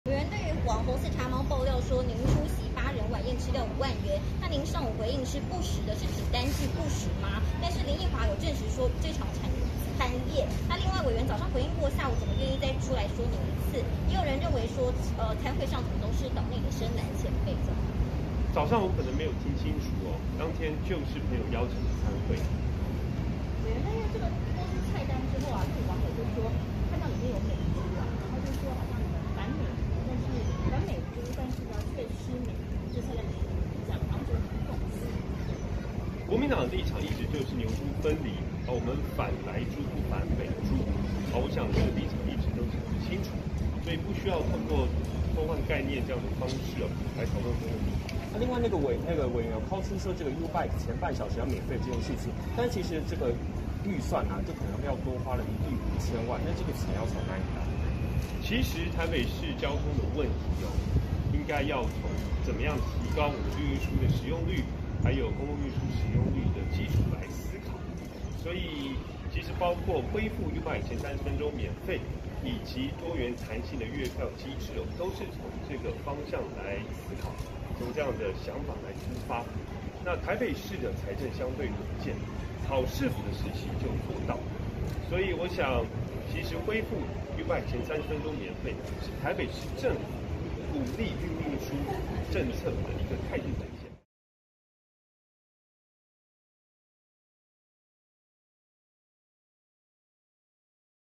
上午批四叉貓爆料8人晚宴5萬「不實」 蔣萬安下午改口證實參加餐會
網紅「四叉貓」劉宇爆料，國民黨台北市長候選人蔣萬安與中廣董事長趙少康等人參加一場晚宴要價5萬多元，蔣萬安今上午在雲林受訪指出，這是不實的內容，應拿正確的資料公布，但蔣萬安競選總幹事林奕華則證實有這場餐會。蔣萬安下午再被問及此事時改口說他沒有聽清楚，並親口證實當天就是參加朋友邀請的餐會。